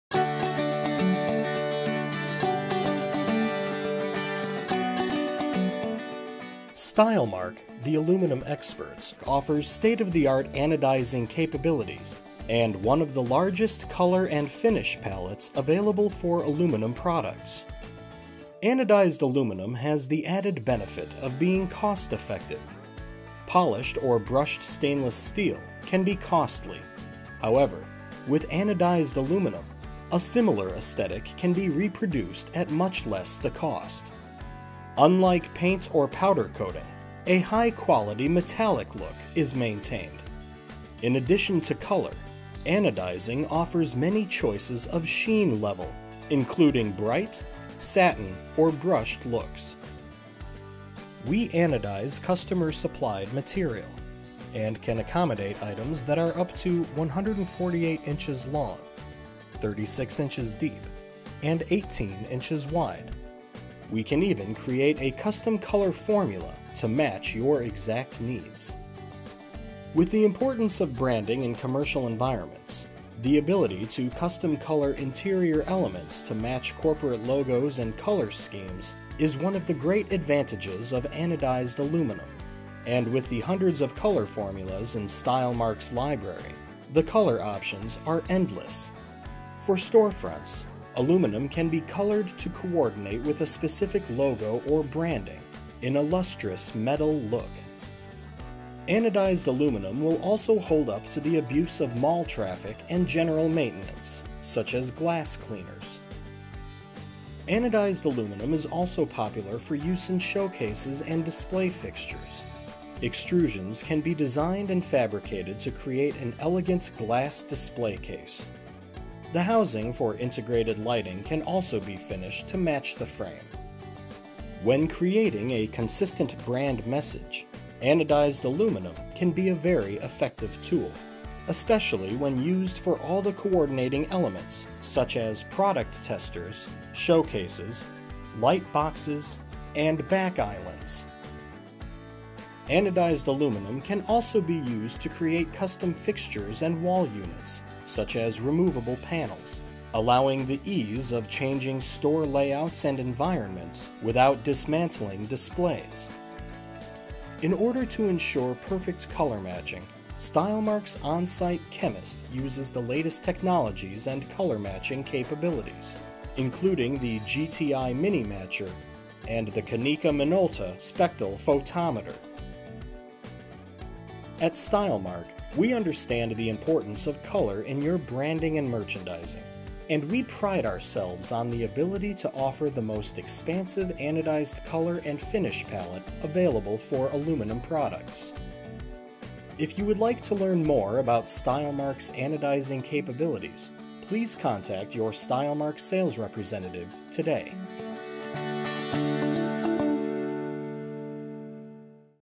Stylmark Anodizing Aluminum Capabilities
Stylmark offers state-of-the-art anodizing capabilities and one of the largest color and finish palettes available for aluminum products. We anodize customer supplied material using one of the hundreds of color formulas in our library, or we can create a custom formula to match your exact needs.